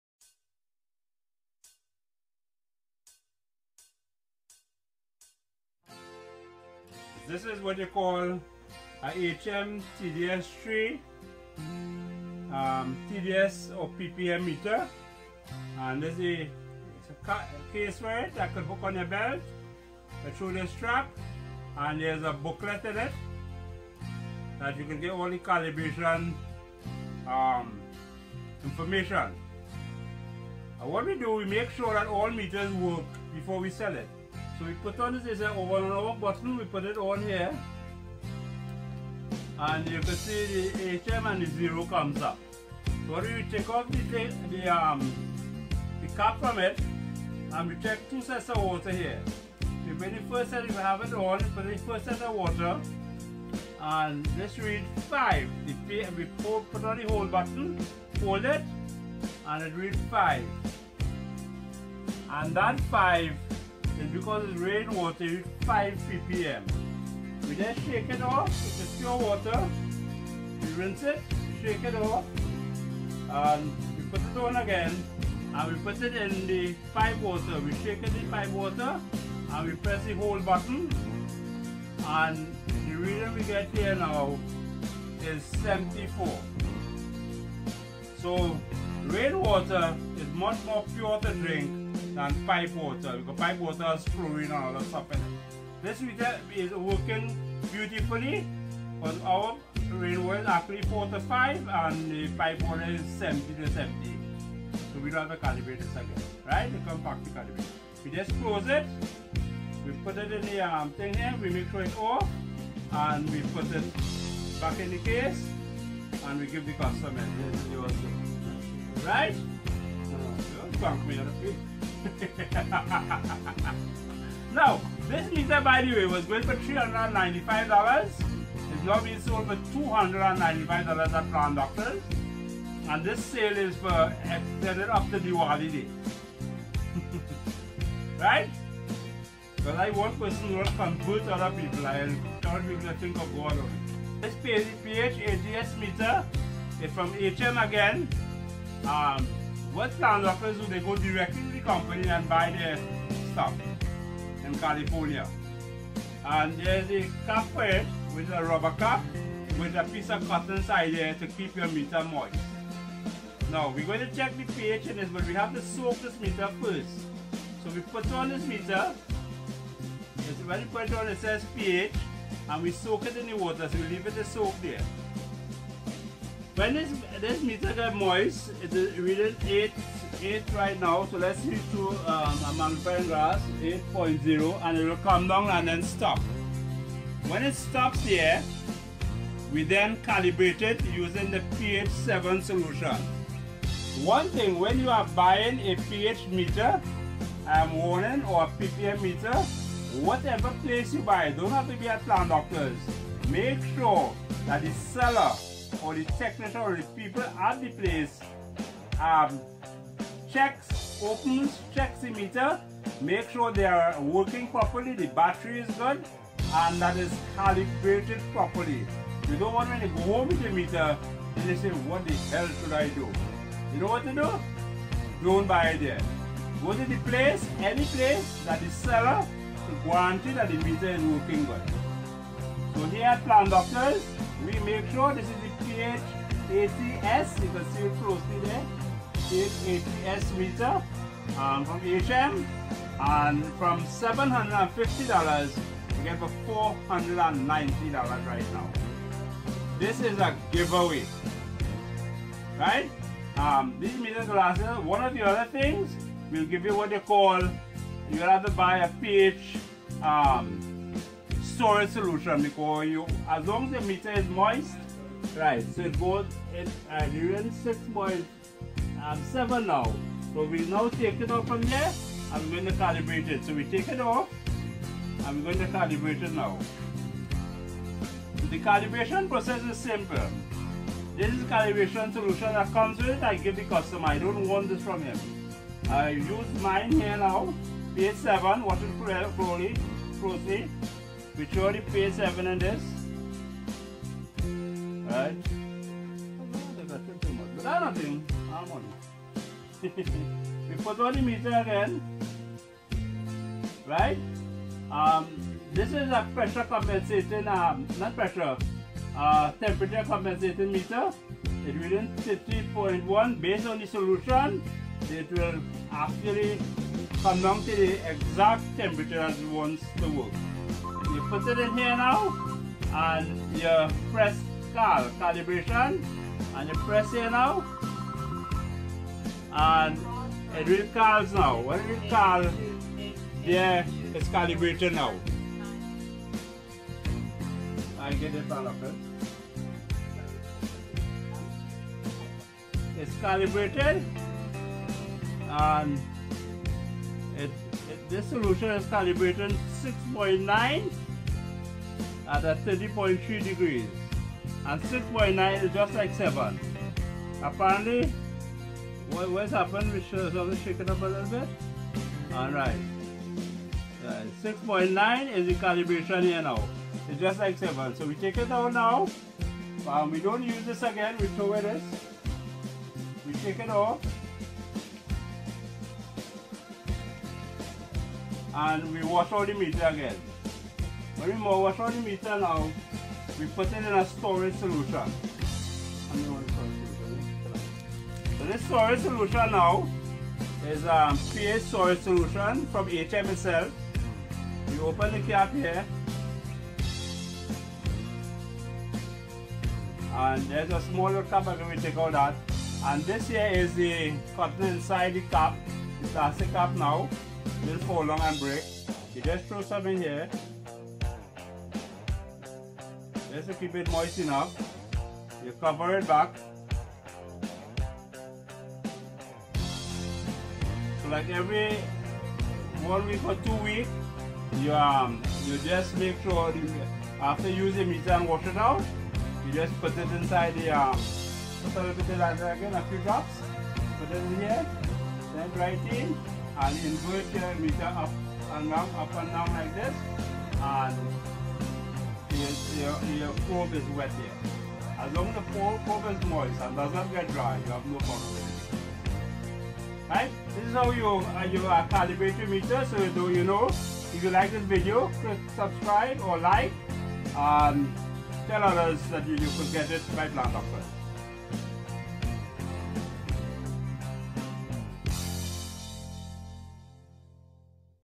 CALIBRATION OF pH AND TDS METERS!!!!
Selling/Calibrating new HM Digital TDS and pH meters

Meters from Video:
PH Meter PH-80 $490
EC/TDS Meter COM-80 $460

7 Canary Street Esperance Philippine, San Fernando